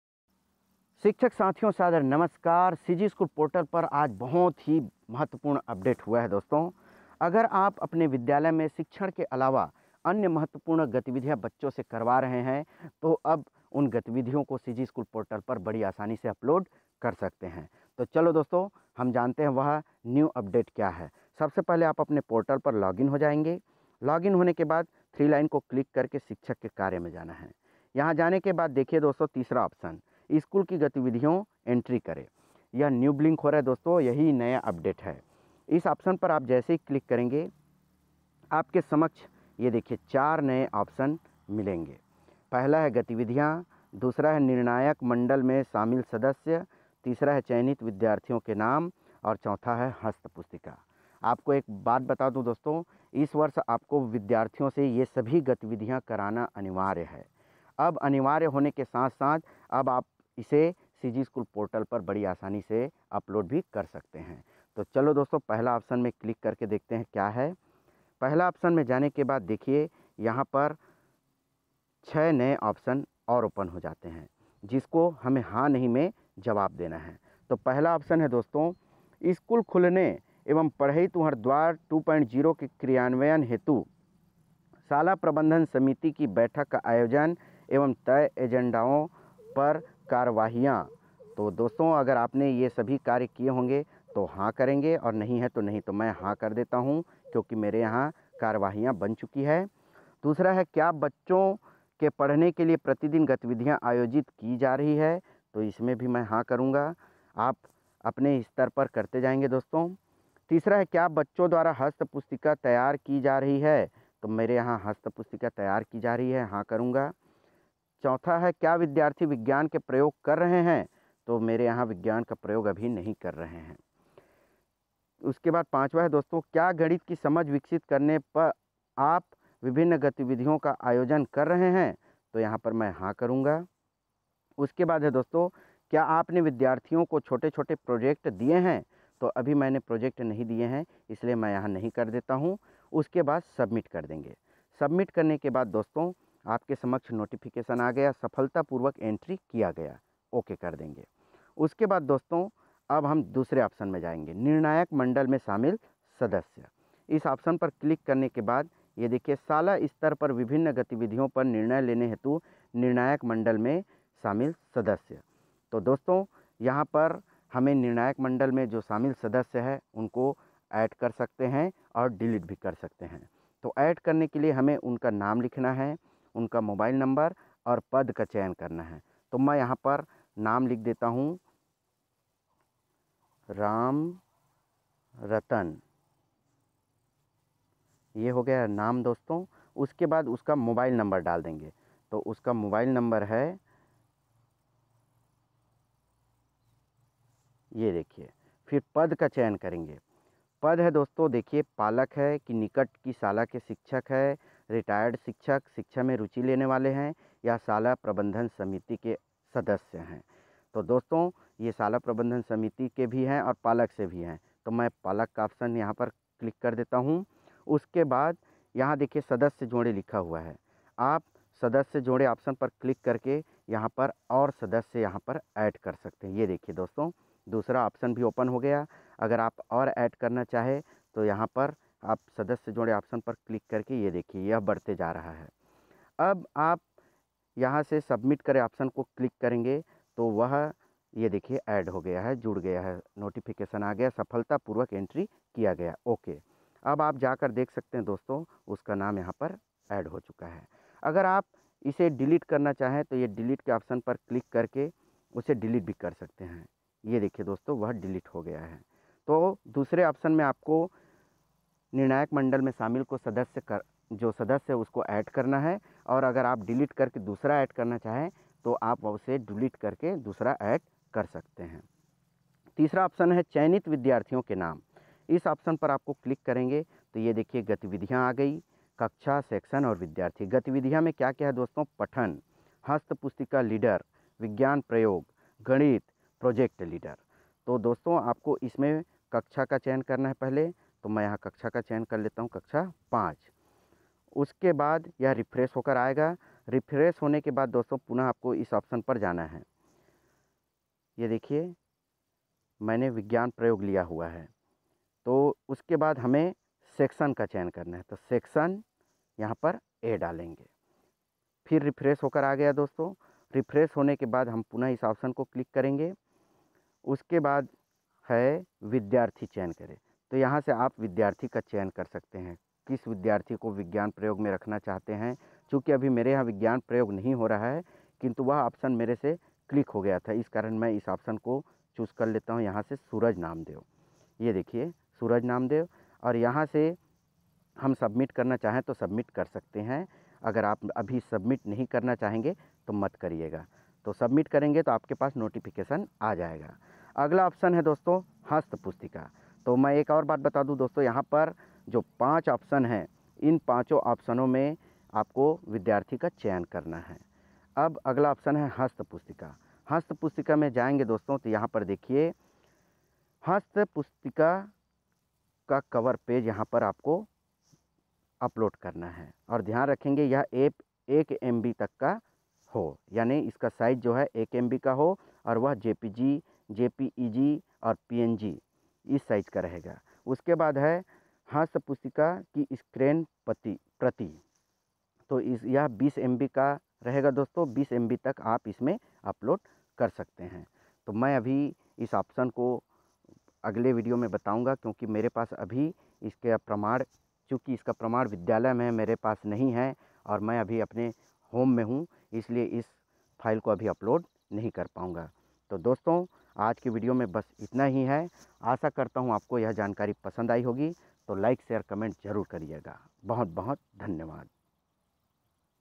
New update in cgschool portal. Apne school ki gatividhiyo ko cg school me upload kare.
upload all activities in your school
all activities uploade in CG school portal
sabhi gatividhiyan CG school portal par upload kaise karen
Apne Vidyalay ki sabhi gatividhiyan CG school portal par upload kare
hanste pustika CG school portal par upload kaise karen